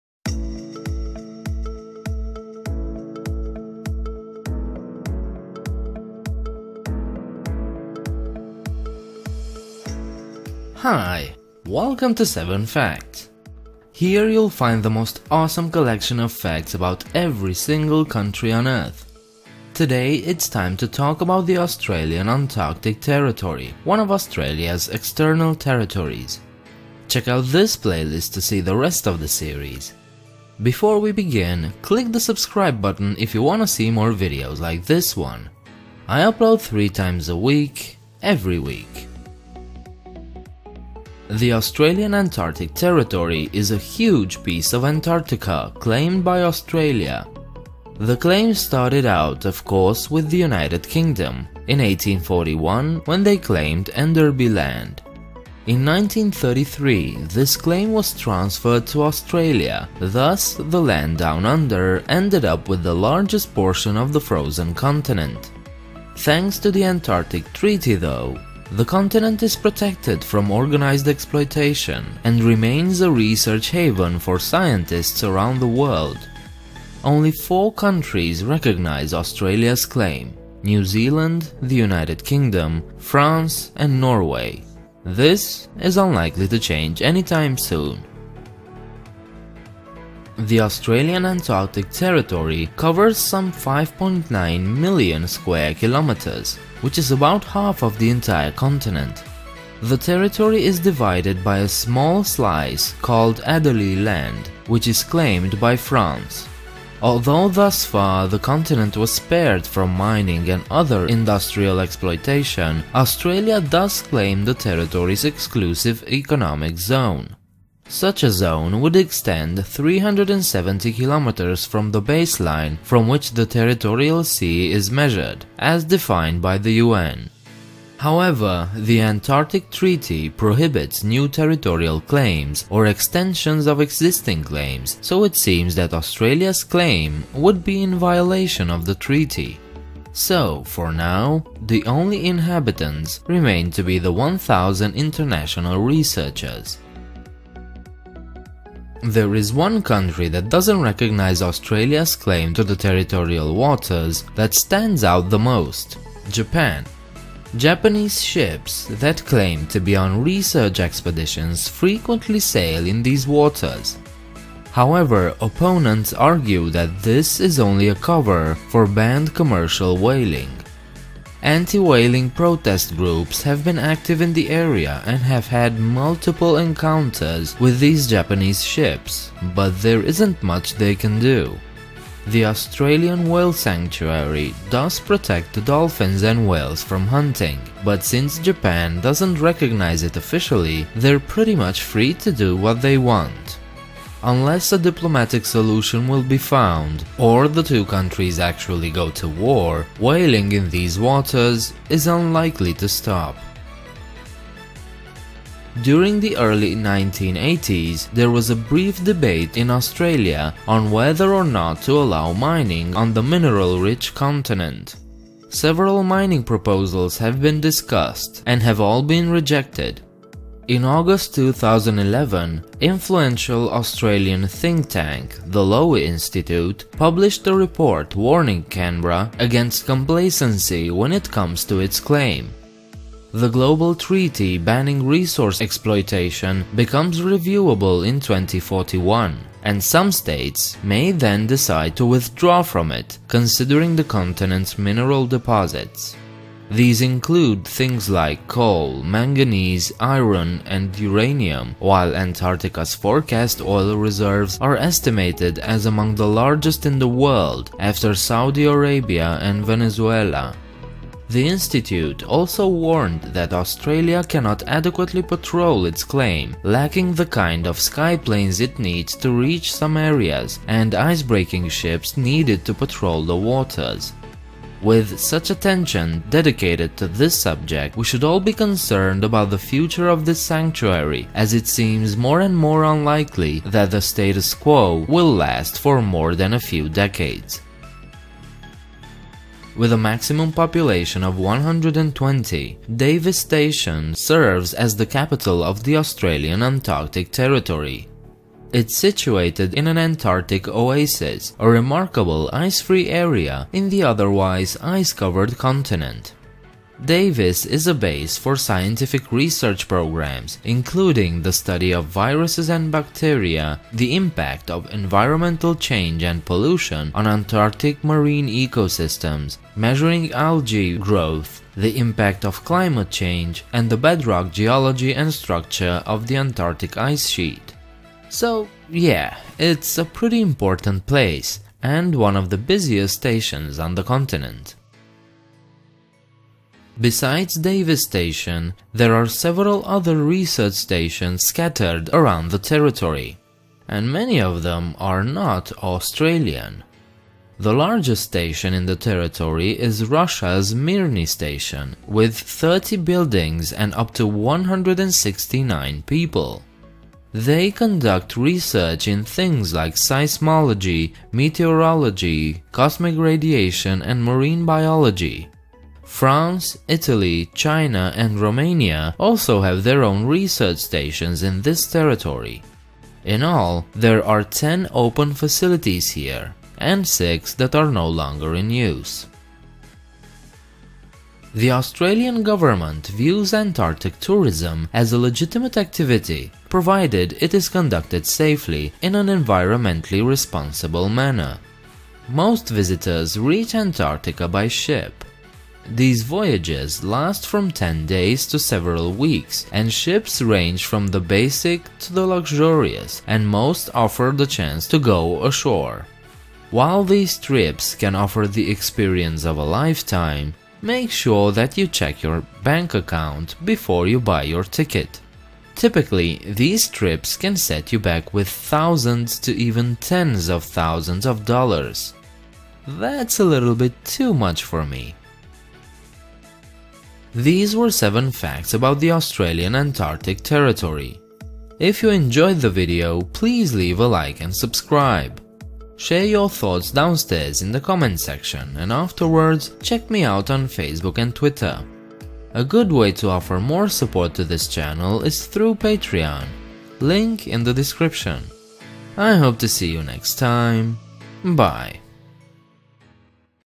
7 Unique Facts about the Australian Antarctic Territory
In this video you can find seven little known facts about the Australian Antarctic Territory.

More information about the video content bellow:
1. The Australian Antarctic Territory (AAT) is a part of Antarctica administered by the Australian Antarctic Division, an agency of the federal Department of the Environment and Energy. The territory's history dates to a claim on Enderby Land made by the United Kingdom in 1841, which was subsequently expanded and eventually transferred to Australia in 1933. It is the largest territory of Antarctica claimed by any nation by area. Only four other countries; New Zealand, United Kingdom, France and Norway recognise Australia's claim to sovereignty in Antarctica.

2. AAT consists of all the islands and territory south of 60°S and between 45°E and 160°E, except for Adélie Land (136°E to 142°E), which divides the territory into Western AAT (the larger portion) and Eastern AAT. It is bounded by Queen Maud Land in the West and by Ross Dependency in the East. The area is estimated at 5,896,500 km2.

3. Whaling in Australian Antarctic territorial waters is controversial and has received international attention. Anti-whaling protest groups, in particular Sea Shepherd Conservation Society, have been active within the Australian Antarctic territorial waters. Sea Shepherd small boat crews have had multiple encounters with Japanese ships that claim to be on research expeditions while opponents argue this is only a "cover" for banned commercial whaling. The Australian Whale Sanctuary, in Australian Antarctic territory, is not recognised by the government of Japan.  Anti-whaling legislation passed by the Australian Government applies to Australian territorial waters. However, Australia's claims of sovereignty over the Australian Antarctic Territory—and thus sovereignty over Australian Antarctic territorial waters—are recognised by only the United Kingdom, New Zealand, France and Norway.

4. During the early 1980s there was a brief debate in Australia on whether or not to allow mining on the mineral-rich continent. Several mining proposals have been discussed and have all been rejected. On the 9 August 2011, influential Australian think-tank, the Lowy Institute, published a report warning Canberra against complacency when it comes to its claim. The global treaty banning resource exploitation becomes reviewable in 2041, and some states may then decide to withdraw from it considering the continent's mineral deposits.

5. The Davis Station, commonly called Davis, is one of three permanent bases and research outposts in Antarctica managed by the Australian Antarctic Division. It serves as the capital of the AAT.

6. Several station operate in this territory and not just from Australia. 5 other countries have research stations in AAT.

7. The Australian Government views Antarctic tourism as a legitimate activity, provided it is conducted safely in an environmentally responsible manner.

Intro Creator:
Pushed to Insanity